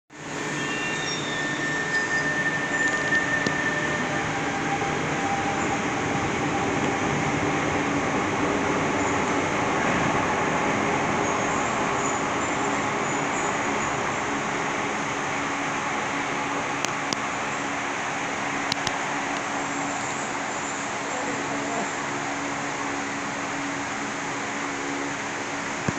Subte Línea A: Formación CNR CITIC 2 Cabina 228 Saliendo De La Estación San Pedrito.
En este video vemos a la Formación CNR CITIC 2 cabina 228 de la Línea A de Subtes de Metrovías saliendo de la estación San Pedrito rumbo a Plaza De Mayo - Casa Rosada.
Composición: Rc228, M228, Mpb228/Mpa228 y Rca228.

Filmado el día Jueves 07/10/2021 a las 21:35 hs.